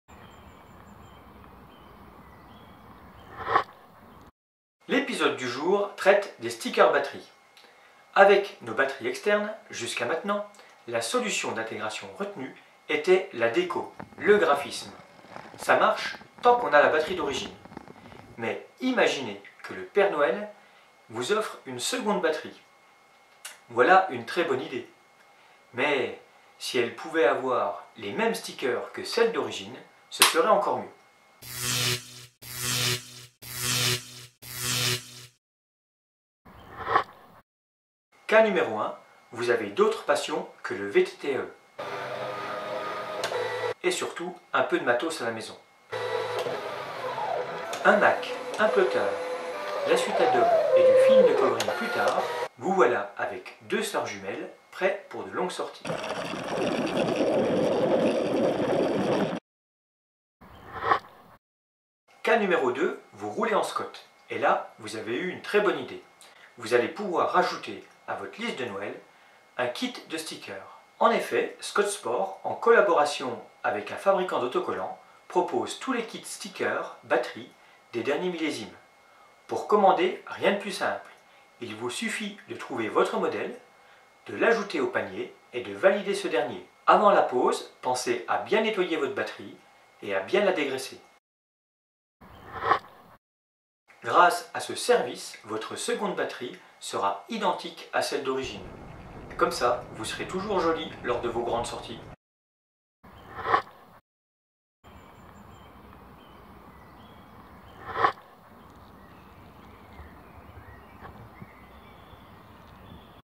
E-Pisode 113 : Sticker batterie SCOTT
Vous roulez en VAE et souhaitez acquérir une seconde batterie ... par contre, celle ci sera neutre ... et vu que sur les premières générations d'E-Bike, l'intégration visuelle étant basée uniquement sur la déco, une batterie toute noire ne serait pas du plus bel effet :(

Quelle solution pour rouler "joli" même sur de longue sortie :)

A bientôt sur les sentiers ...